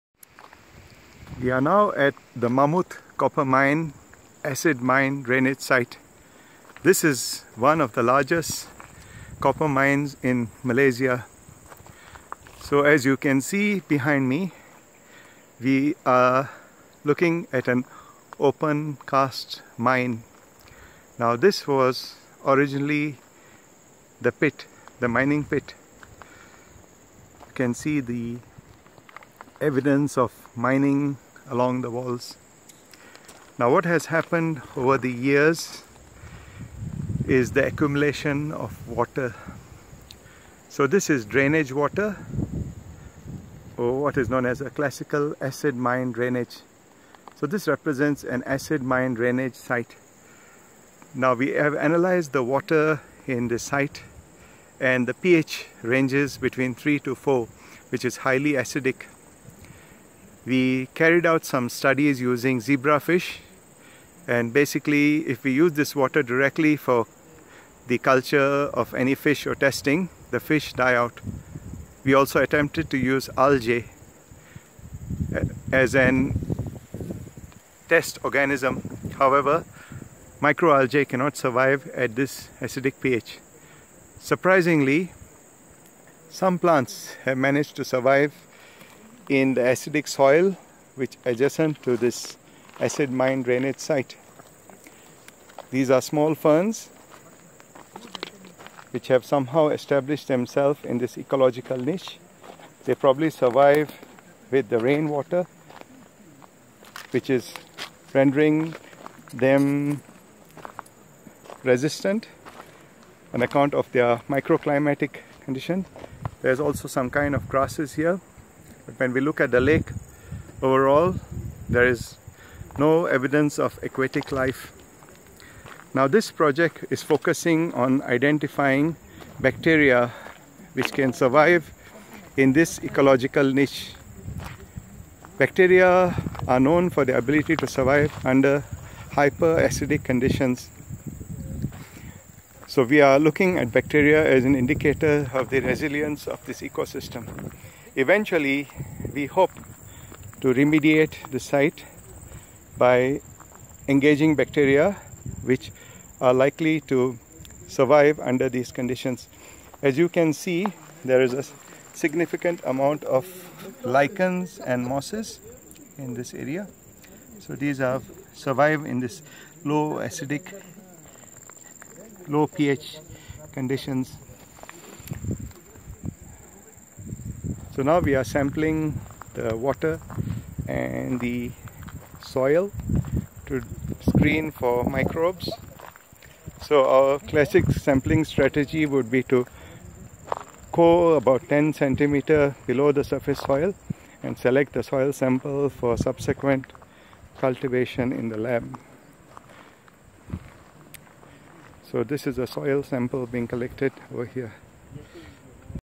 Mamut Copper Mine
This is the Mamut Copper Mine, Acid Mine Drainage site in Sabah, Malaysia. The mine ceased operations in the 1990s and what remains behind is a giant pit filled with runoff from the surrounding cliff faces. The low pH of the water (3.0 to 4.0) and the high concentration of Copper in the soil do not permit the growth of algae or terrestrial plants. However, recent observations have indicated that some resilient species have adapted to this extreme ecological niche. Our research work is focusing on the isolation of copper tolerant microbes and we have isolated a variety of Bacillus which can thrive in the acidic conditions and possess the necessary genes for adaptation to Copper.

Acknowledgements: Jabatan Mineral dan Geosains Malaysia ,Sabah

Funding from: Ministry of Education, Government of Malaysia, FRGS 0455